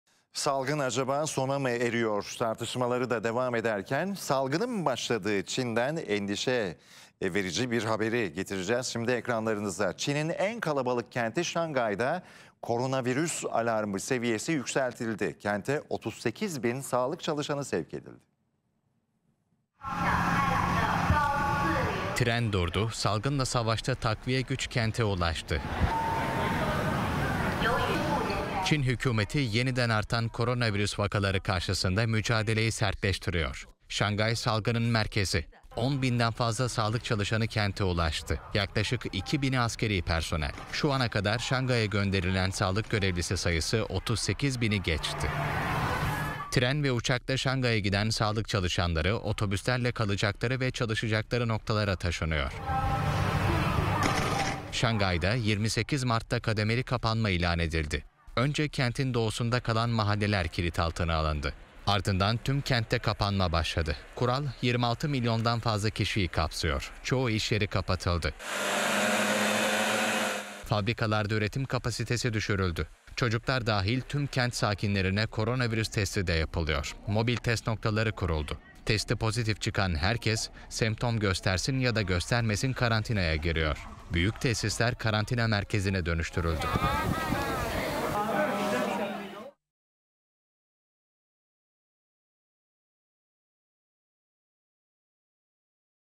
Bir Tren Dolusu Doktor... Şangay'da Neler Oluyor?
Çin hükümeti Şangay'a Covid-19 pandemisiyle mücadele için 38 bin sağlık çalışanı ve askeri personel yardımı gönderdi.